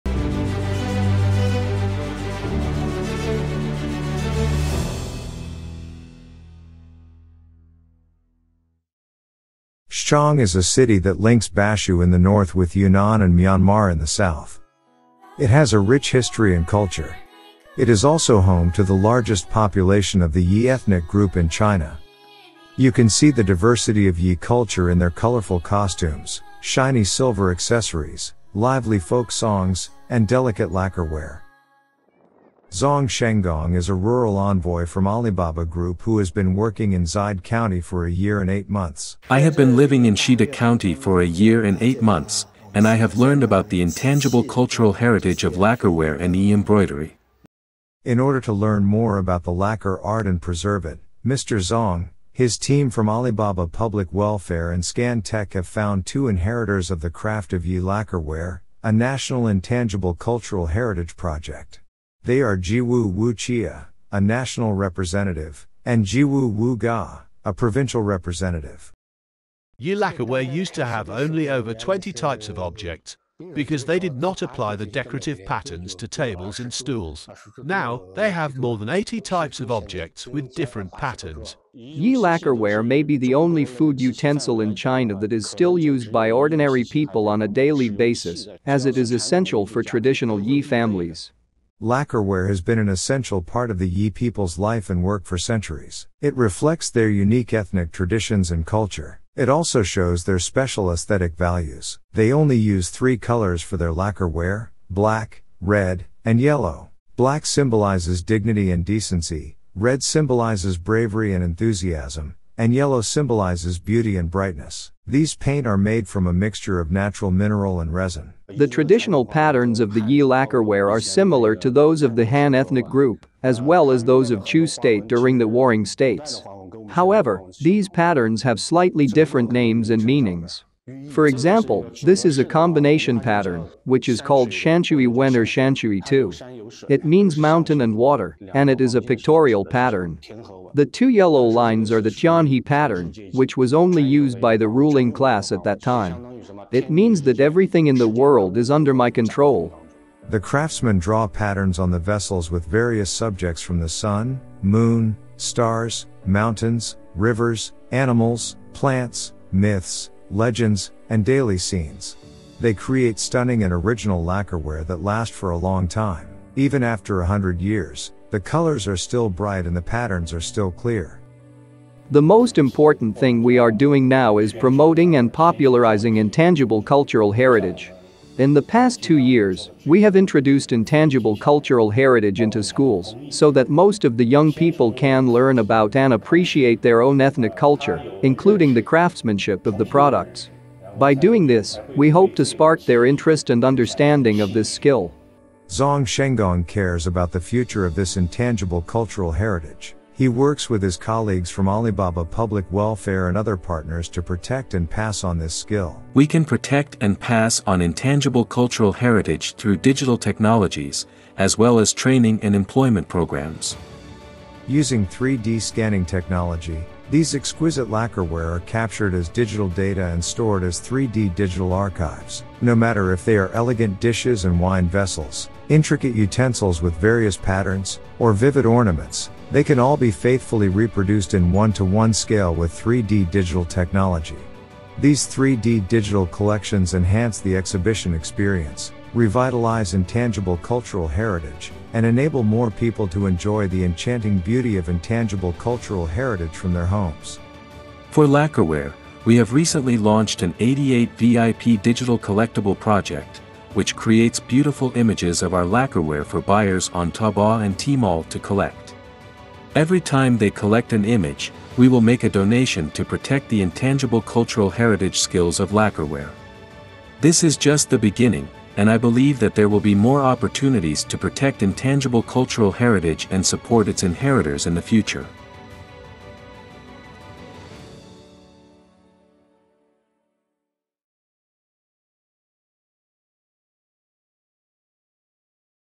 How 3D Scanner Helps to Save the Yi Lacquerware, a Precious Intangible Cultural Heritage
Lacquerware is an intangible cultural heritage that reflects Chinese Yi ethnic group’s unique traditions and aesthetics. They use natural paints and draw various patterns on the vessels, creating stunning and original artworks that last for a long time.✨✨

Join us in exploring how 3D scanning preserves this enchanting beauty of intangibleculturalheritage. 🙌🙌